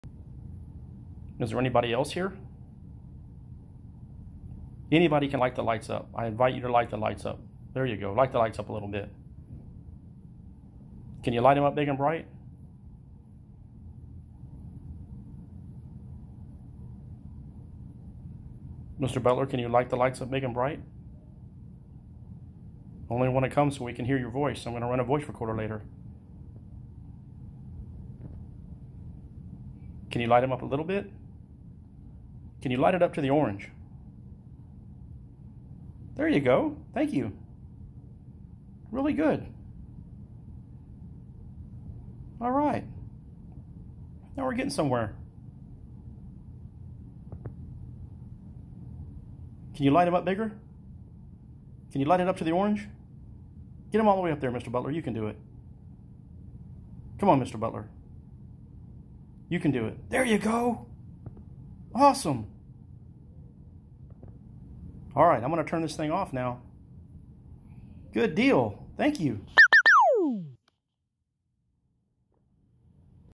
Battle House Hotel K2 Hit
Made possible contact with Mr Butler who was murdered on the 5th floor. Notice how the K2 goes nuts when I say I'm turning it off.  Then spikes at the end.  Here's the history.